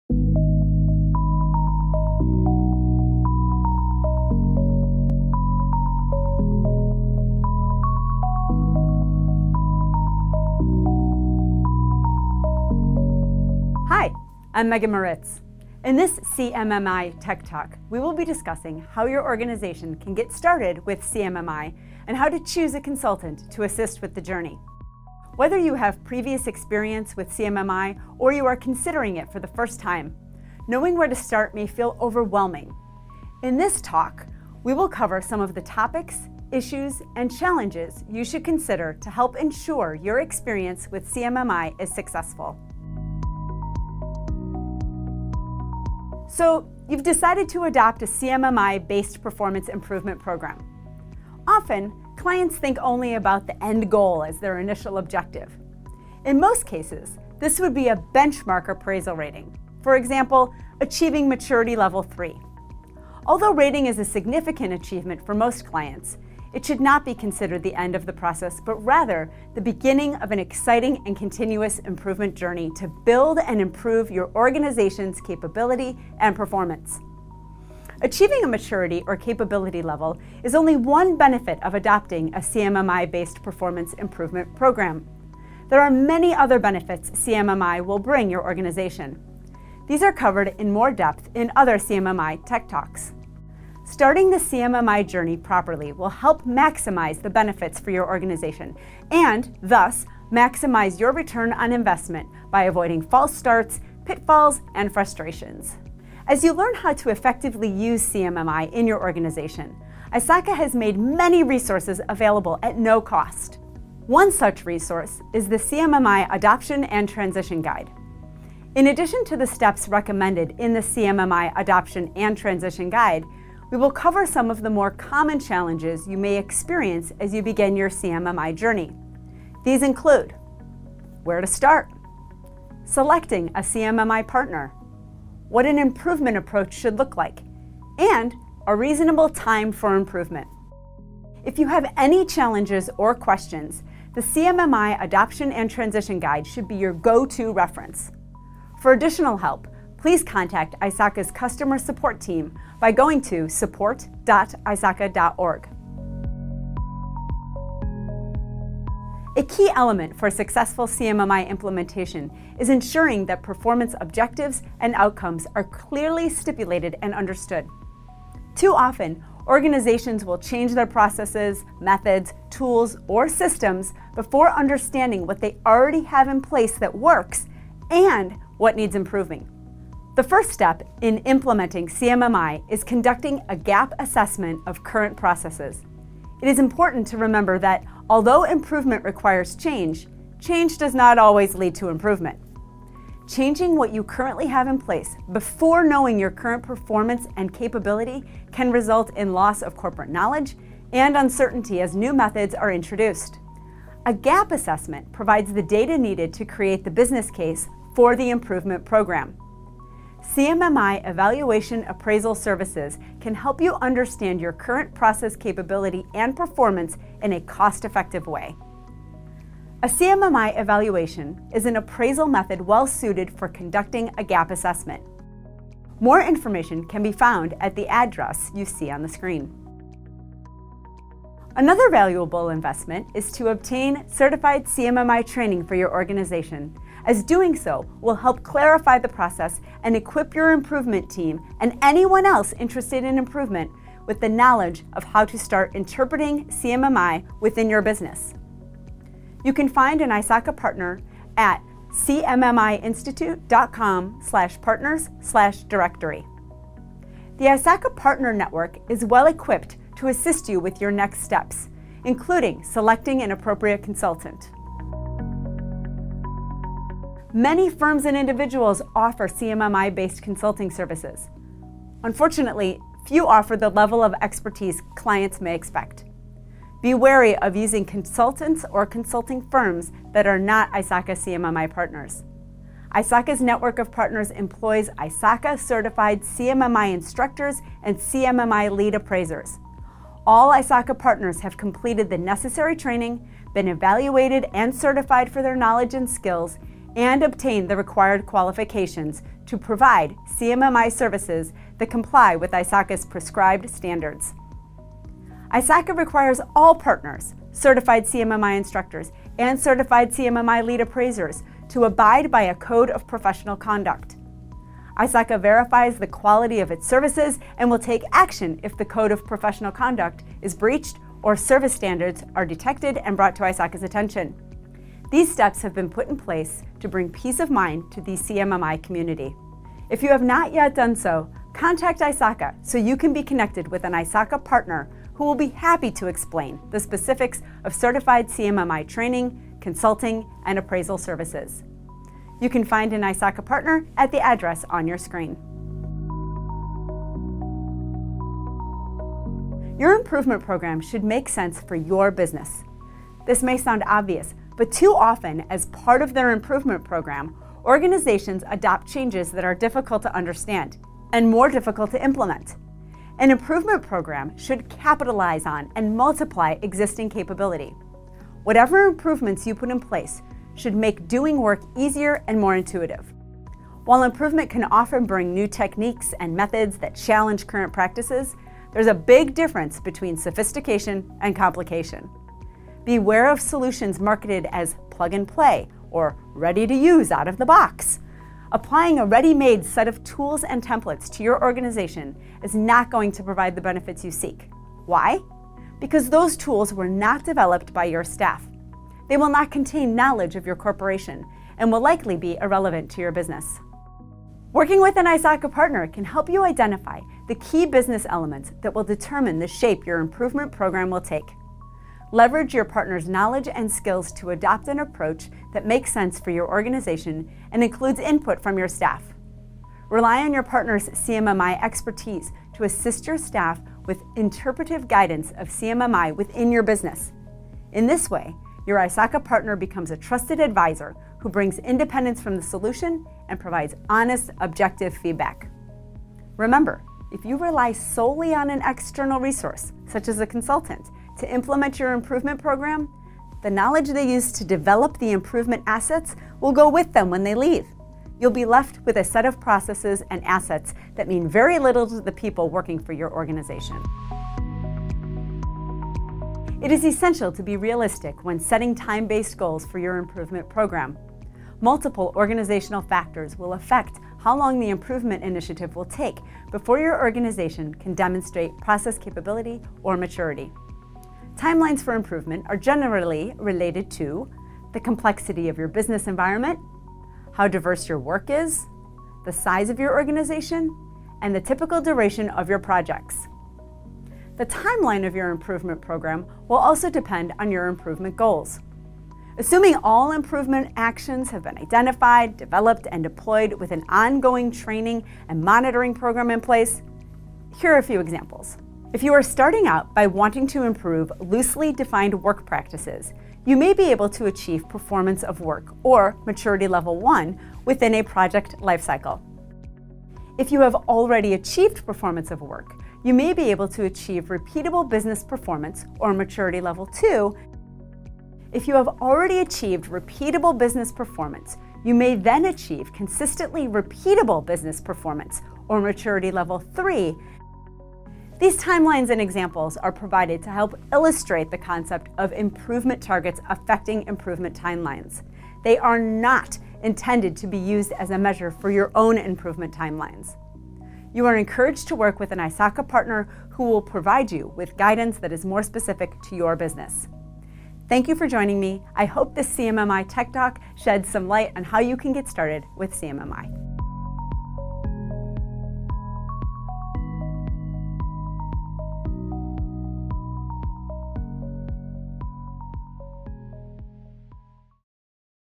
How to Get Started with CMMI
How to properly get started with CMMI adoption and how to choose a reputable consultant to help your organization on its performance improvement journey.